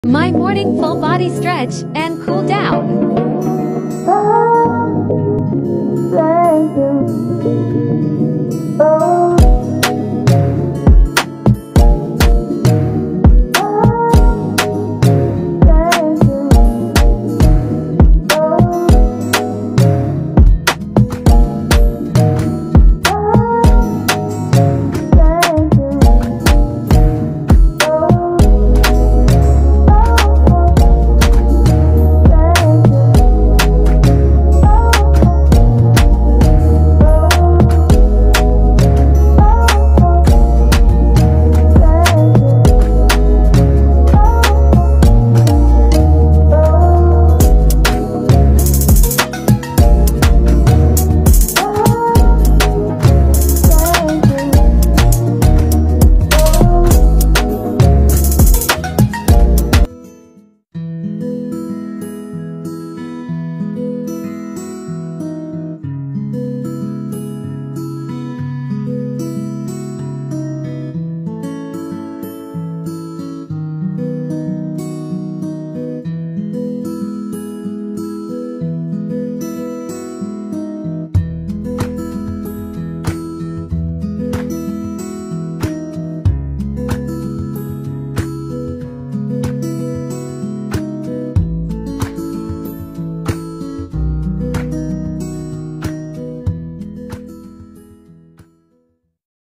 My Morning Full Body Stretch & Cool Down.
Whatever problem you face,  Remember you're "Blessed".

Recently, I enjoyed doing 20mins Morning Yoga (Full Body Flow/Stretch for Beginners) by @madfit.ig . After, I added my own cool down, it helps me think, be relaxed and positively move every day. 🤍🧘‍♀️